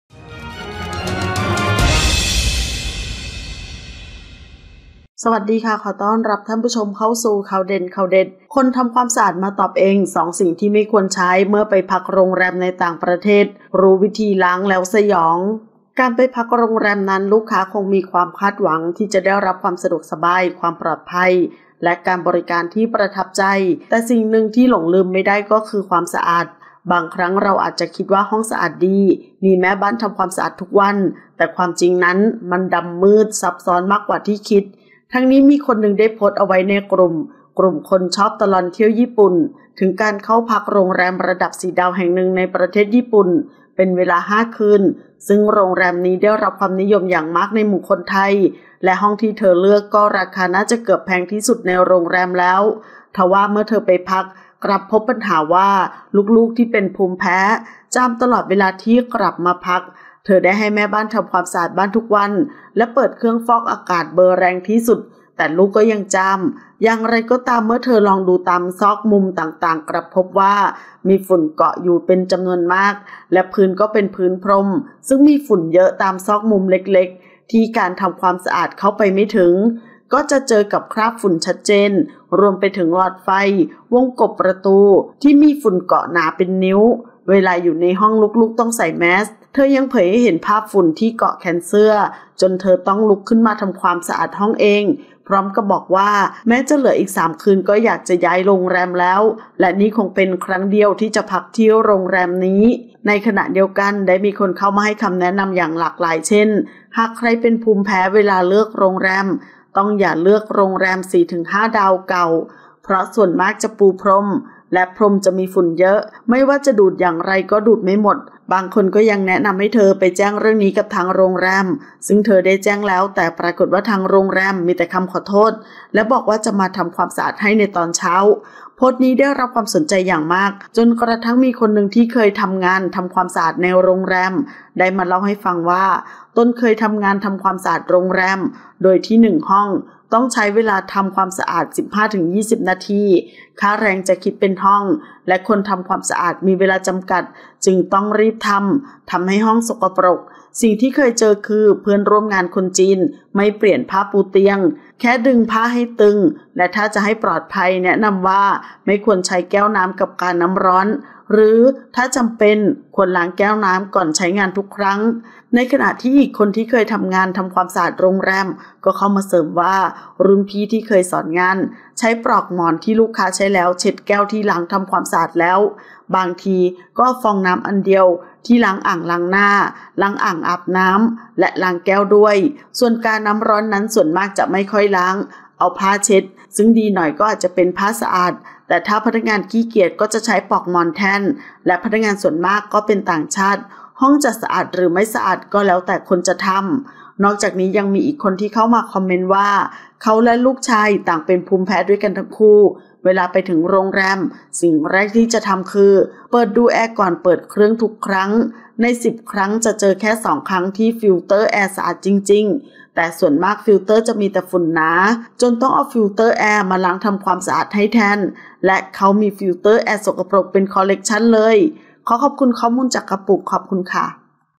คนทำความสะอาดมาตอบเอง 2 สิ่งที่ไม่ควรใช้ เมื่อไปพัก รร. ในต่างประเทศ รู้วิธีล้างแล้วสยอง!
ขอขอบคุณข้อมูลจาก: kapook

00:00 - Intro
00:10 - News
01:00 - Minute